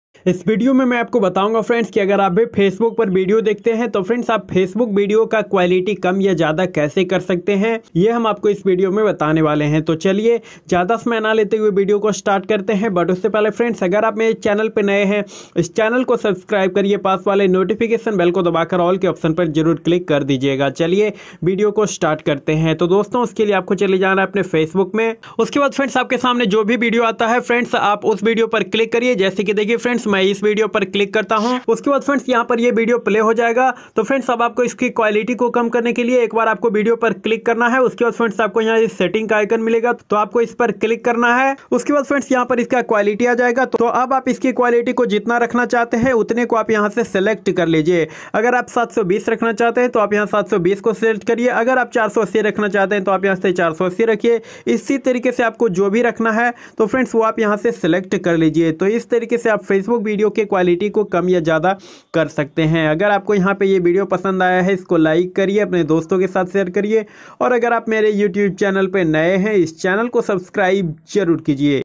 Facebook video ka quality kam aur jyada kaise kare।। how to select Facebook video quality
is video me bataya gaya hai friend ki Facebook video ka quality kam ya jyada kaise kiya jata hai